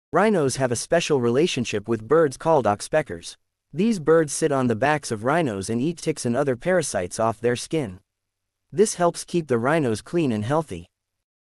Oxpeckers and Rhinos
Rhinos have a special relationship with birds called oxpeckers. These birds sit on the backs of rhinos and eat ticks and other parasites off their skin. This helps keep the rhinos clean and healthy.